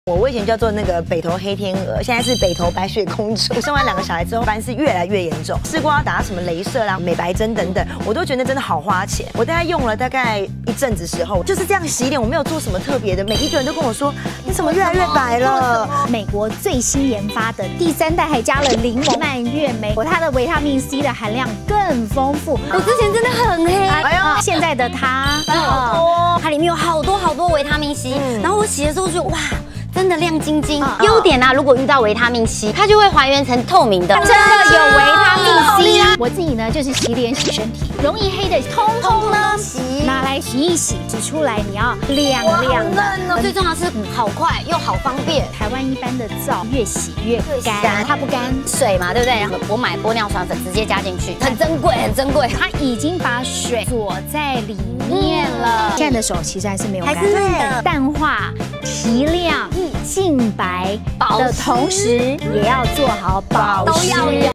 569195#213840#美國超級A+淡斑皂 真的可以边洗澡边美白 全家适用 天然无核珍珠 淡斑效果极佳
美国进口淡斑皂
回头率第一
真的可以边洗澡边美白 全家适用
天然无核珍珠 有效溶解黑色素 深层净白 淡斑效果极佳